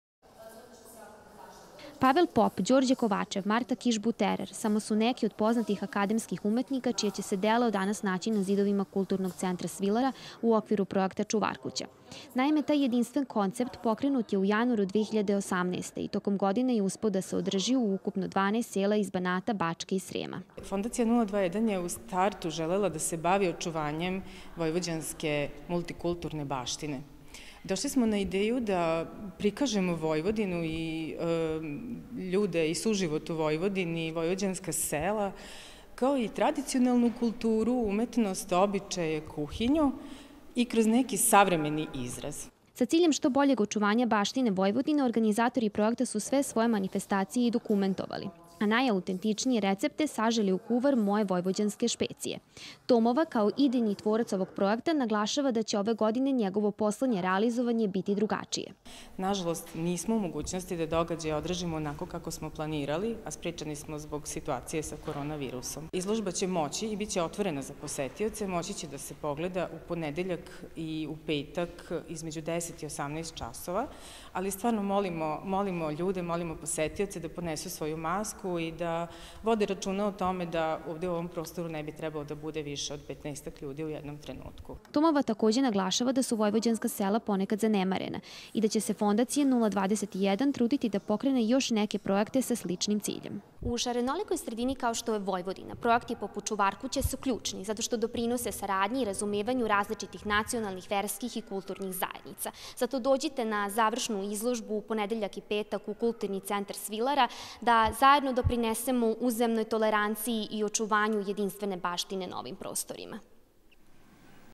Završna izložba u okviru projekta "Čuvarkuća" u Svilari
Projekat "Čuvarkuća", u okviru Fondacije 021, svoje dvoipogodišnje trajanje zaokružuje izložbom u novosadskoj Kulturnoj stanici Svilara.